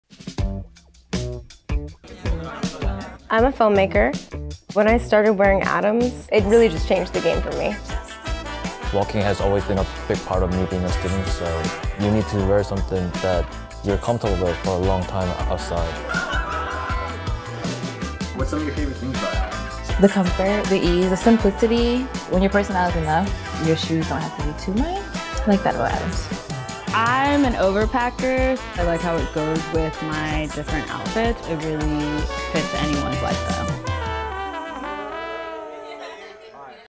Give the gift of comfort
Thoughtfully crafted to pair a simple and versatile design with materials that maximize comfort, Atoms sneakers are perfect for anyone on your holiday list.

Follow Atoms!